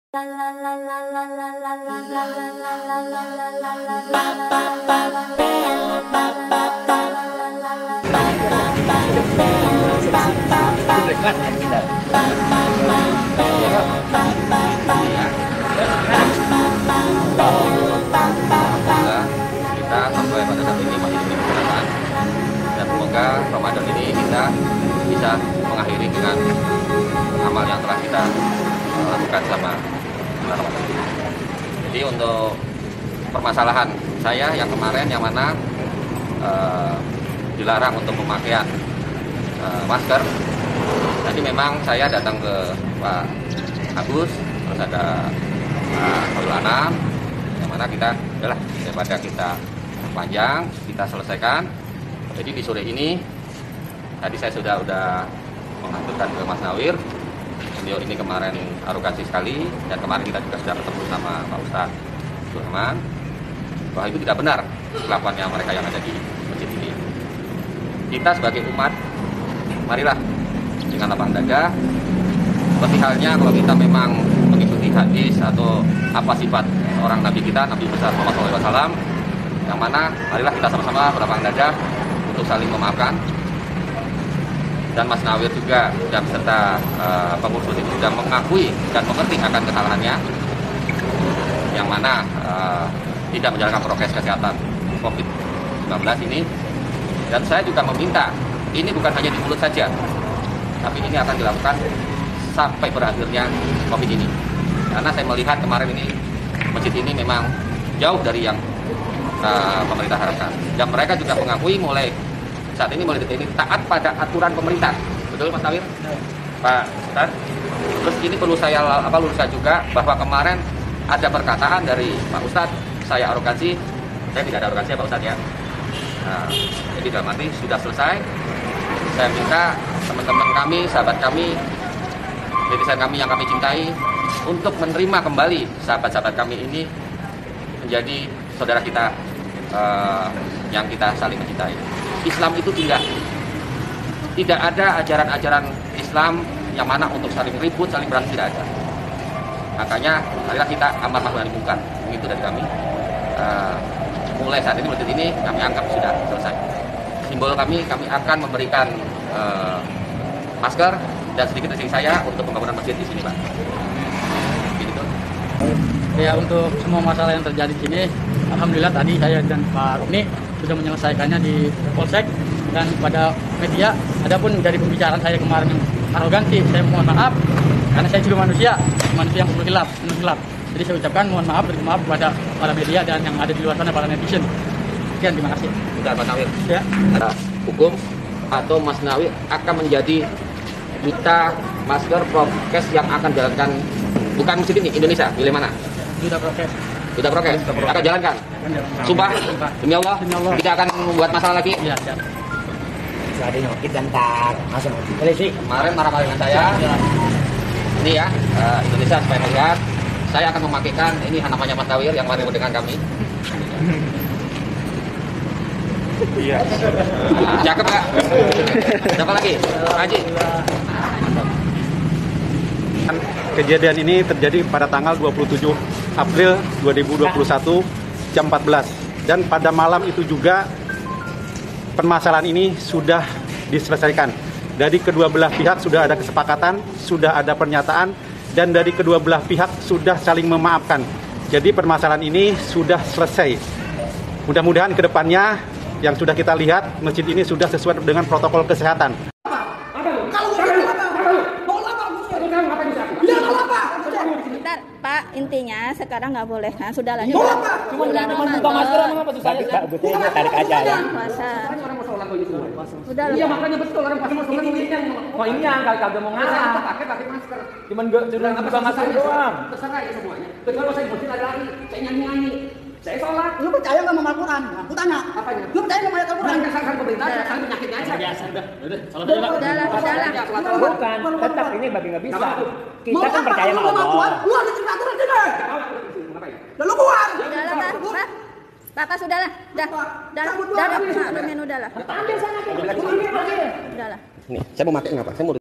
Nawir remaja masjid yang tarik masker jemaah jadi Duta Masker
Nawir (23), sosok pemuda masjid yang menarik paksa masker Roni Octavianto (46) menjadi duta protokol kesehatan (prokes). Nawir menjadi duta prokes usai menyampaikan permohonan maaf ke Roni.

Roni mengatakan, dirinya sudah bertemu dengan Nawir untuk menyelesaikan persoalan pada peristiwa dia diusir hingga maskernya ditarik paksa di Masjid Al Amanah Bekasi yang viral beberapa waktu lalu.

Roni  menawarkan kepada Nawir untuk menjadi Duta Prokes agar kedepannya Nawir dapat memberikan contoh kepada Masyarakat Indonesia.

"Mas ada dua pilihan, Mas Nawir memilih saya (proses) secara hukum, atau Mas Nawir akan menjadi duta masker prokes yang akan jalankan di tidak hanya masjid tapi indonesia?," tanyanya kepada Nawir di Bekasi, Rabu (5/5/2021).

Nawir langsung menjawab pertanyaan itu dengan yakin.

Duta prokes, sumpah, demi Allah," jawab Nawir.

Dia mengatakan, akan memberikan contoh yang baik agar tindakan yang telah dia lakukan tidak terulang kembali di daerah lainnya.

"Alhamdulillah dipercaya jadi duta masker (prokes) jalankan sebaik mungkin supaya tak terulang lagi. Dan saya akan menjalankan sebaik mungkin prokes dari pemerintah," janjinya.